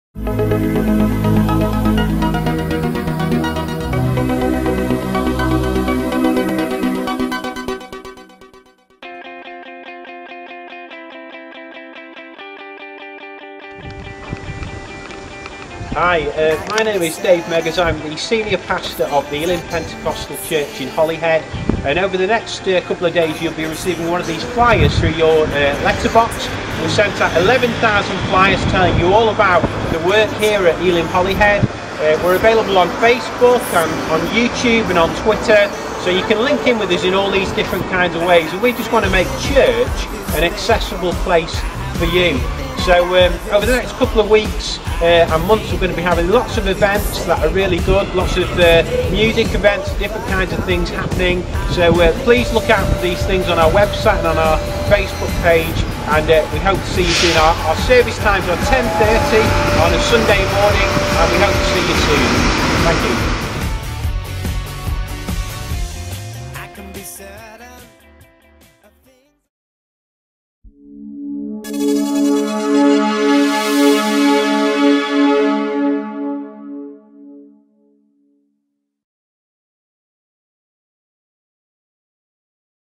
Big issue no.2: Leaflet Drop
Elim Holyhead has recently dispatched over 11,000 flyers throughout the town of Holyhead.